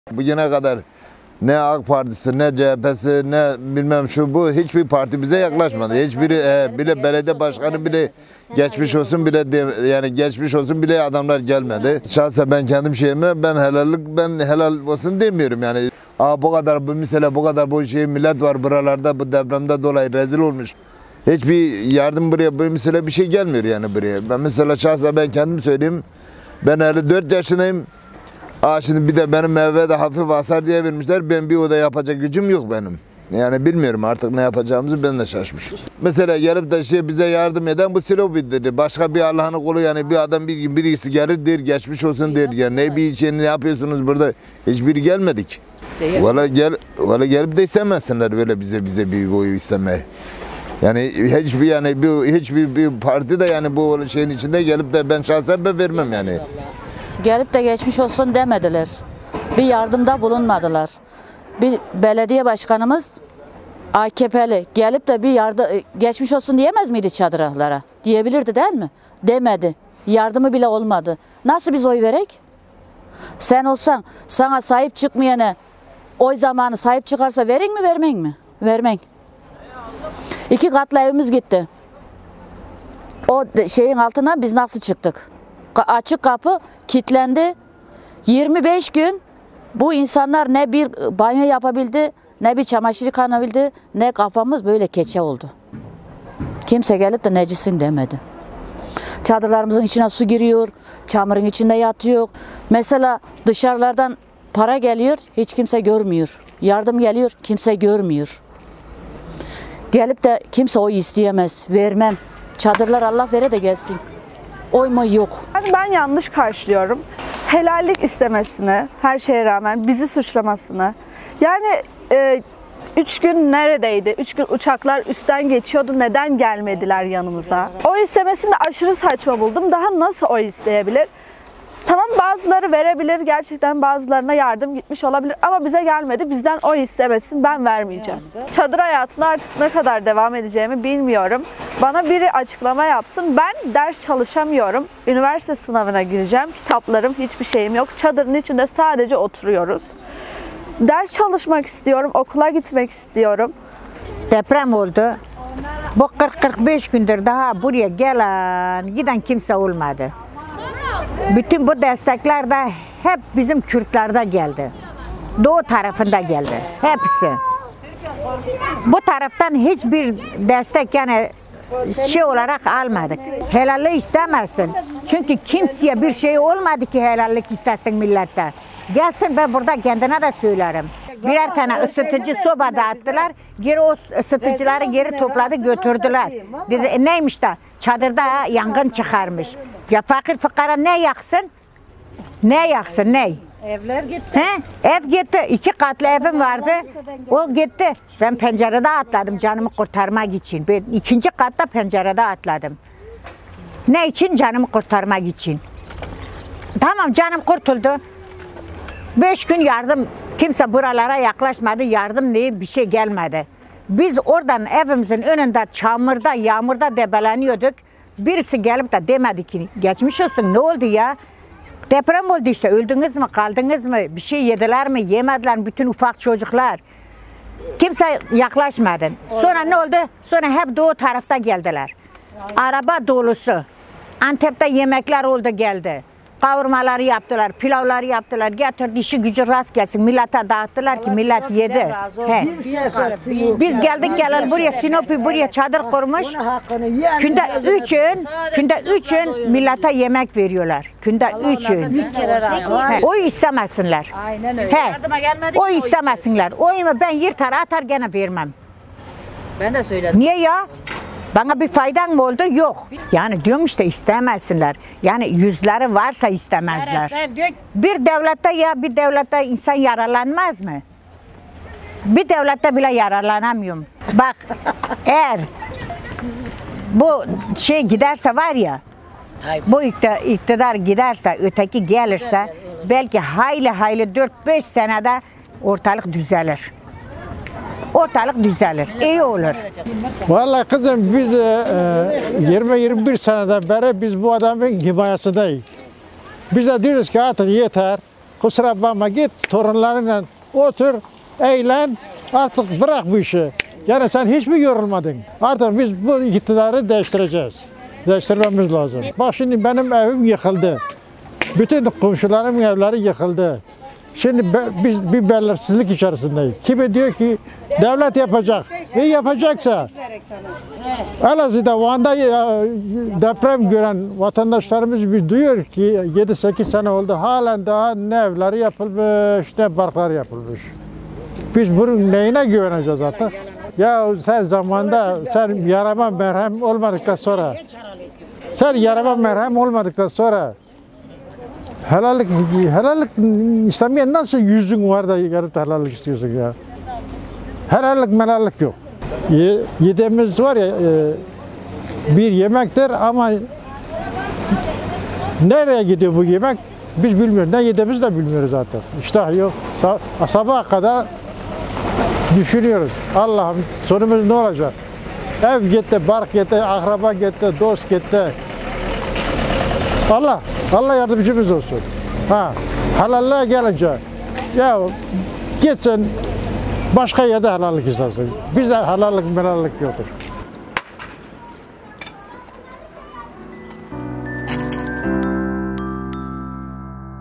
DEPREMZEDELER: 20 YILDIR İKTİDARDA, ARTIK GİTSİN
Mereş merkezli depremin üzerinden 50 gün geçmesine rağmen depremzedelerin ihtiyaçları hala karşılanmış değil. AKP’li Cumhurbaşkanı Recep Tayyip Erdoğan ise, “Bu ölçekteki afetler karşısında eldeki imkanlar yetersiz kalıyor. Uluslararası yardım çağrısında bulunduk” itiraflarında bulunarak, gittiği deprem bölgesinde önce helallik, ardından oy istedi. Öte yandan gittiği her yerde yeni konutlar yapacaklarına dair açıklama yapan Erdoğan'ın bu açıklamalarına depremzedeler tepki gösterdi